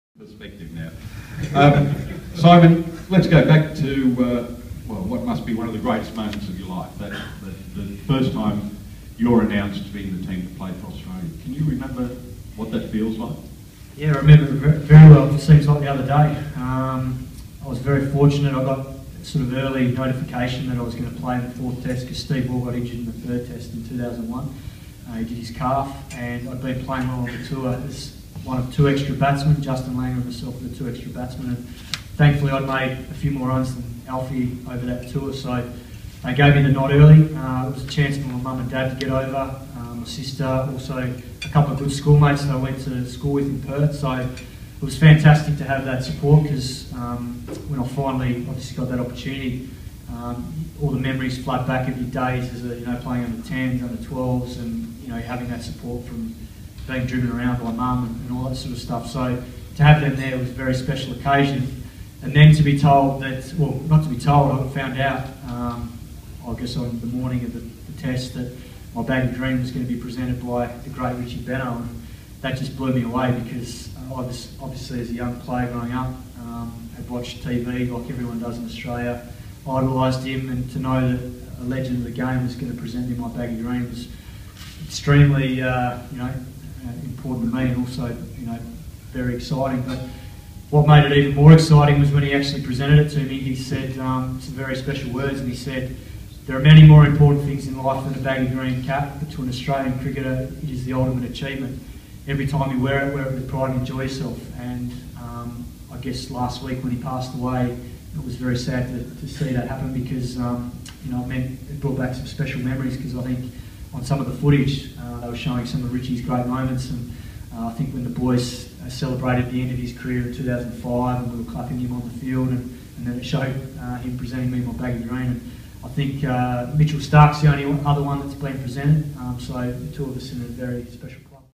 Royal Canberra Golf Series - Simon Katich - Recalling His Test Debut
Receiving his "Baggy Green" from the great Richie Benaud